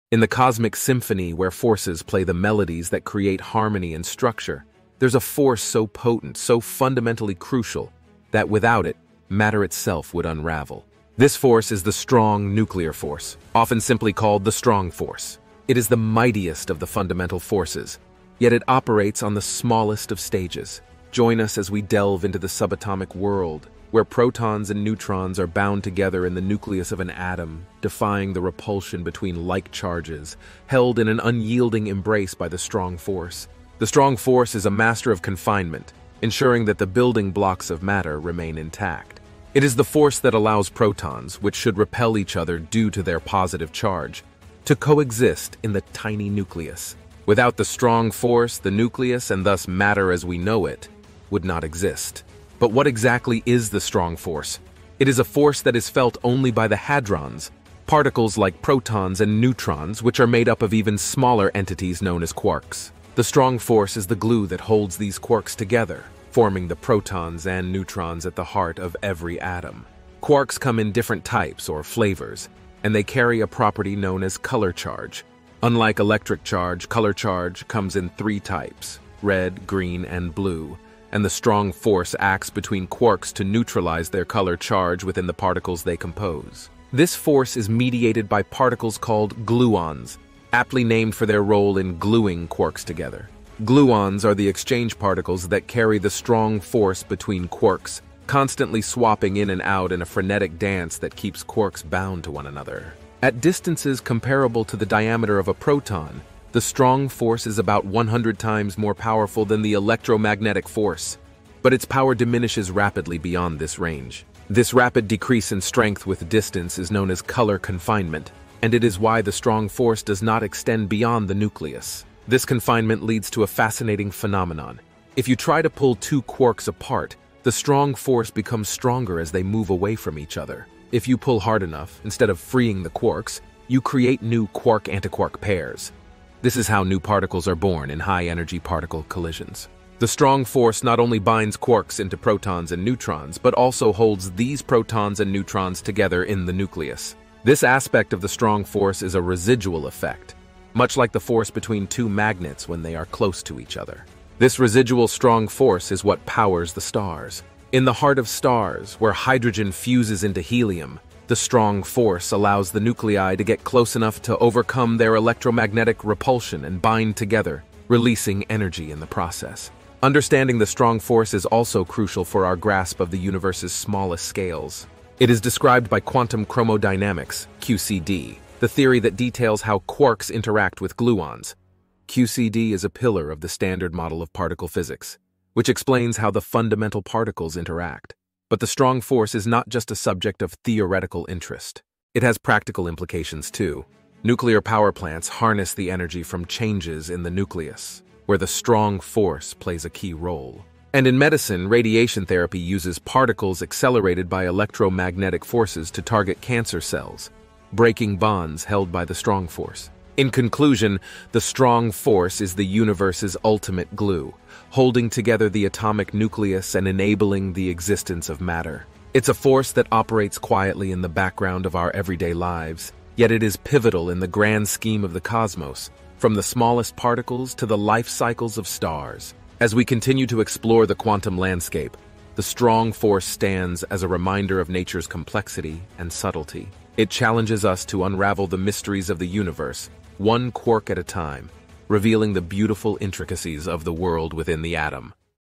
The Strong Force: Understanding the Hidden Power Inside the Atom
Embark on a subatomic adventure to discover the strong force, the invisible power that holds the universe's matter together. In this video, we dive into the heart of the atom to explore how this fundamental force operates, binding quarks to form protons and neutrons, and ensuring the stability of the atomic nucleus against the repulsive might of electromagnetism. Learn about color charge, gluons, and the complexities of quantum chromodynamics that define the strong force's role in the cosmos, from the formation of elements within stars to the technologies we use every day.